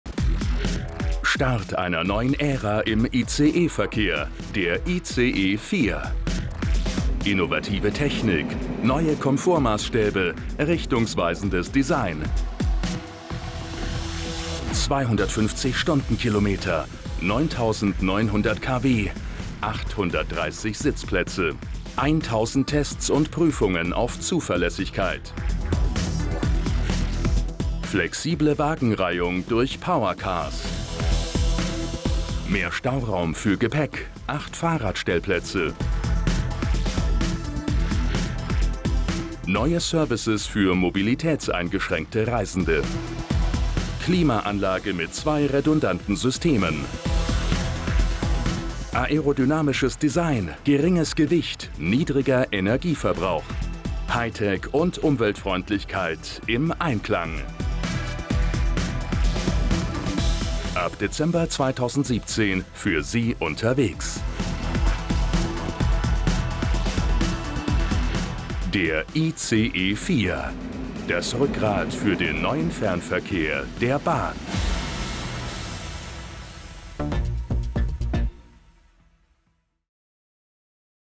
Der ICE 4 kommt!
Dürfen wir vorstellen? Der ICE 4! Mit ihm startet eine neue Ära im ICE-Verkehr der Bahn.  Der Hochgeschwindigkeitszug wird im Dezember 2017 seinen Regelbetrieb aufnehmen.

Mit einer Gesamtlänge von 346 Metern und einer Höchstgeschwindigkeit von 250 Stundenkilometern bietet der zwölfteilige ICE 4 insgesamt 830 Sitzplätze. Er  besteht aus zwei Endwagen, einem Restaurant, acht Reise- sowie einem Servicewagen mit Kleinkindabteil, Familien- und Rollstuhlbereich.  Sein Energieverbrauch liegt bei einem Leergewicht von 670 Tonnen gegenüber einem redesignten ICE 1 mit zwölf Wagen und zwei Triebköpfen um 22 Prozent pro Sitzplatz niedriger.

Mehr Informationen und Fakten zum ICE 4 sowie Grafiken und Bildmaterial zum Download